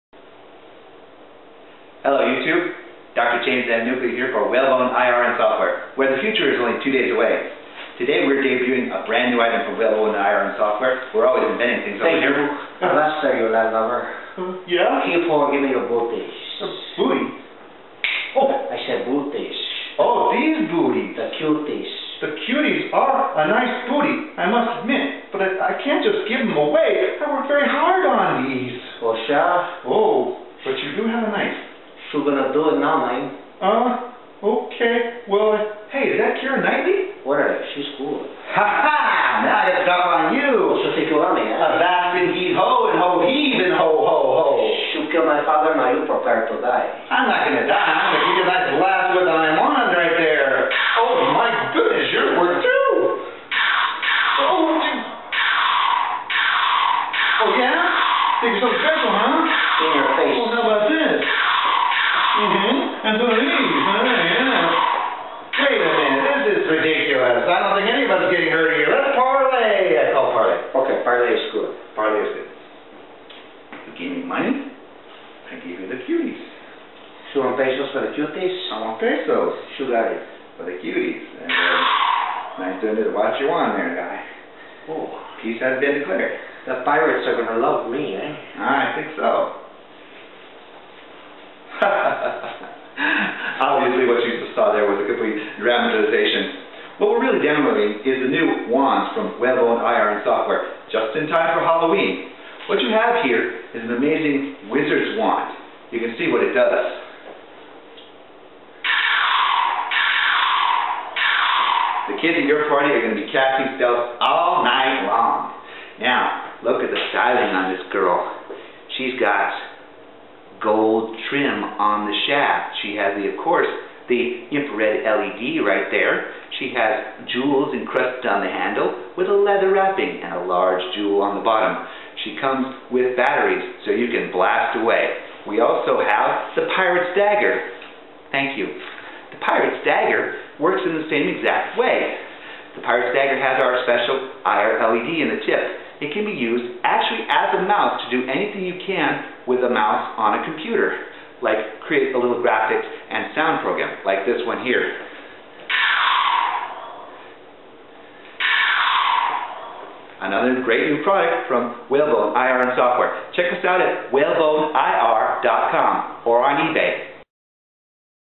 Wand and Prirate IR pens Smart board alternative - Halloween props -WBSW
For more info. and demos, checkout my other videos or go or search for "IR pen" on ebay.

Happy Halloween every body. Please enjoy this Little skit that we cooked up and consider purchasing a Wizard's Wand IR pen, or the Pirate's Dagger IR pen. The wand shooting effect that you see in this video were created using Processing and the Wii Interactive Whiteboard system. The Kid's Cuties IR sources are also a real product from Whalebone IR and Software. We will be creating a full video about them soon too.